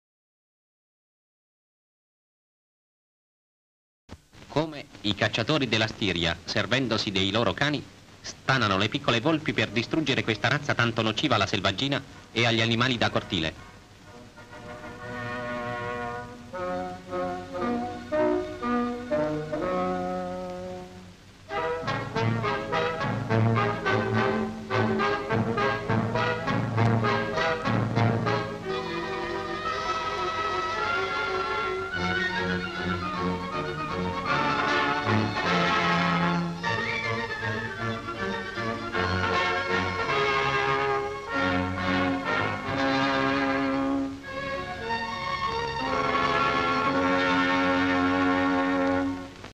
Cacciatori stanano volpi
Descrizione sequenze:cacciatori con cani nei boschi della Stiria ; i cacciatori stanano piccoli di volpe dalle loro tane ; i cacciatori si allontanano con i piccoli di volpe catturati e i cani al seguito ;